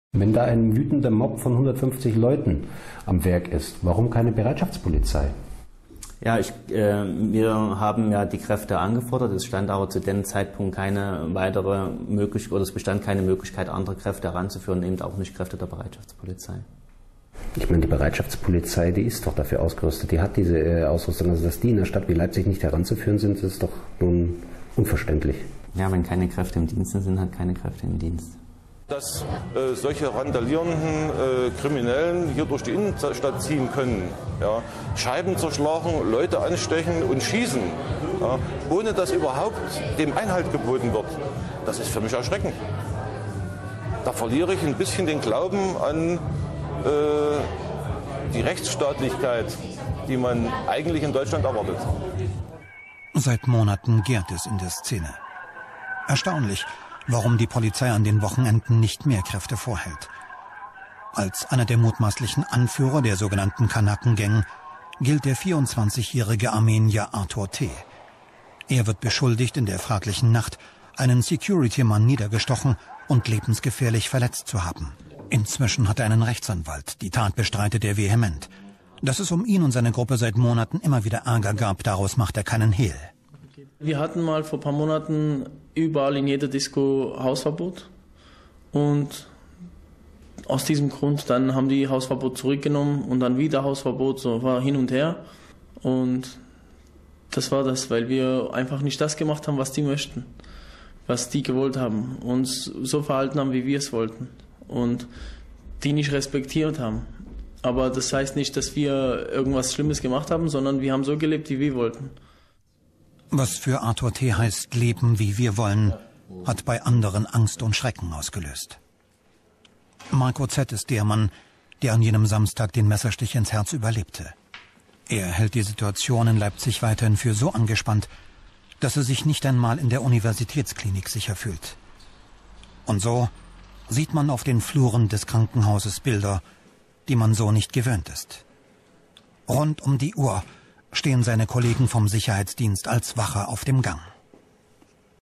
Der Armenier Artur Terowakimjan
Die iranisch-armenische Drogenmafia und die Blutnacht von Leipzig.